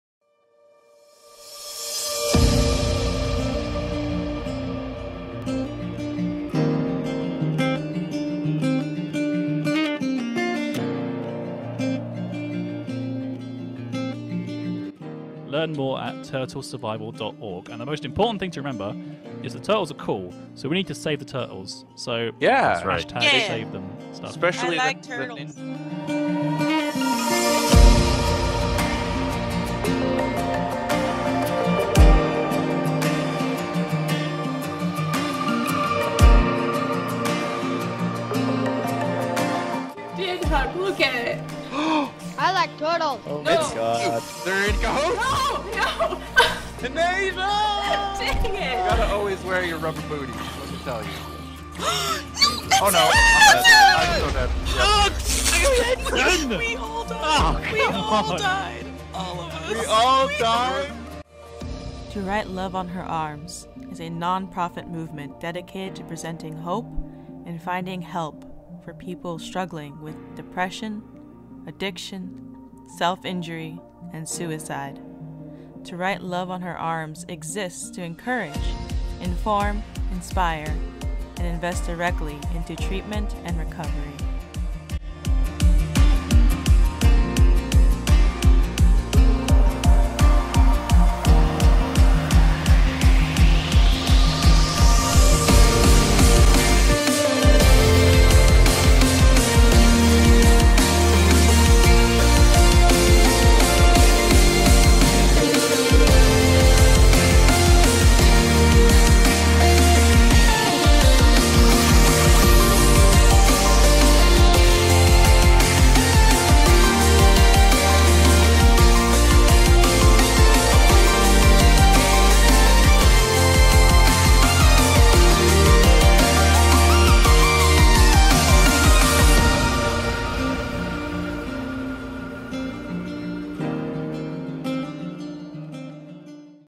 TimeShot SMP \\ Charity Live Stream Announcement! \\ April 9, 2016 \\ TWLOHA
Timeshot SMP is having their 2nd Annual Charity Live Stream event on Saturday, April 9th. Last year we raised $1600 with your guy's help for the Turtle Survival Alliance. This year we're raising funds for TWLOHA: To Write Love On Her Arms. They're a nonprofit organization that seeks to help and assist those struggling with depression, addiction, self-injury and suicide. They have an amazing story, so please check them out and if you feel you need help, they can assist you.

We're streaming from 8am EST to 8pm EST on April 9th. Make sure to check our twitters or streaming channels (Twitch or Beam) for more information on who is streaming when.

TimeShot members

Special Thanks To - BrendanDRenders for TimeShot Renders